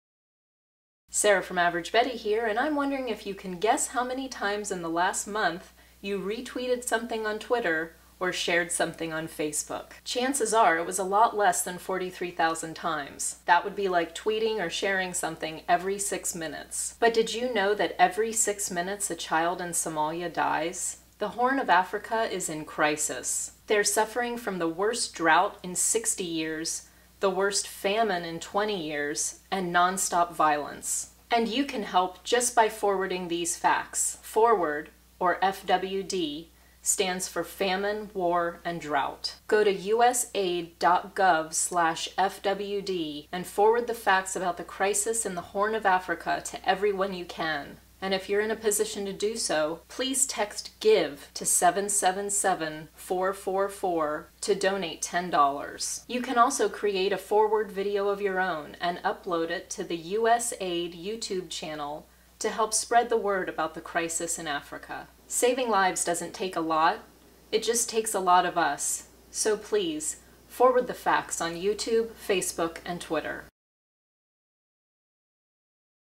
FWD this Video: There's a Famine in the Horn of Africa
Over 13.3 million people are in crisis from Famine, War and Drought in the Horn of Africa. But there is hope.

Text "Give" to 777444 to donate $10 to be a part of the solution.